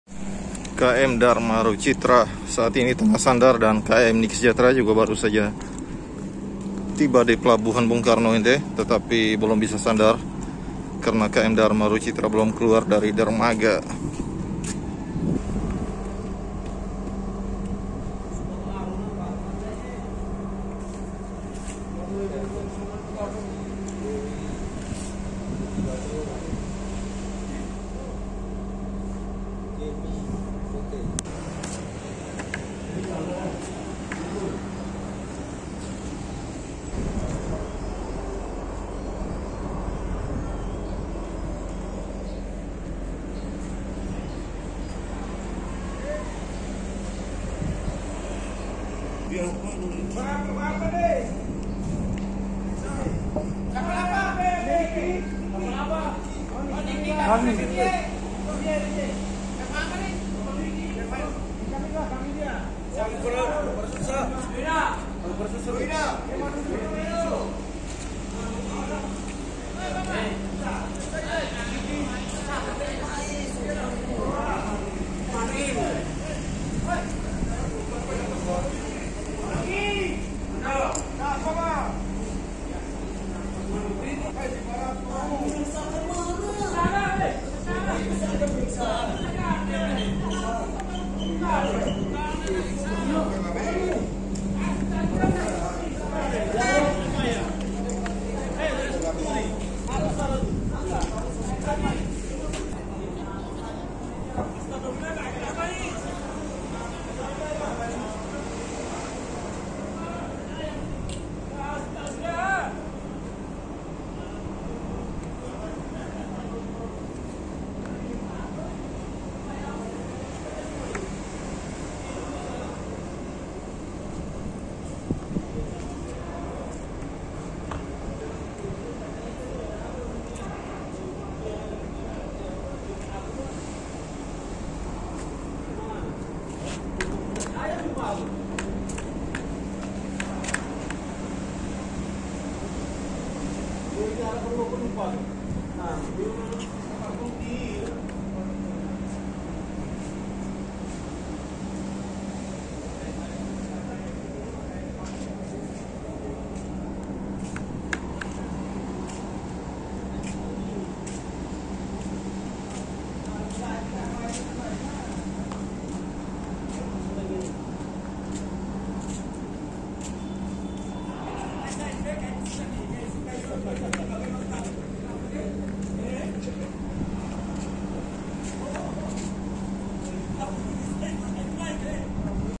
KM NIKI SEJAHTERA & KM DHARMA RUCITRA 8 ANTRI SANDAR DI PELABUHAN ENDE
Ende
pelabuhan ENDE
km niki sejahtera
km dharma RUCITRA viii
kota ende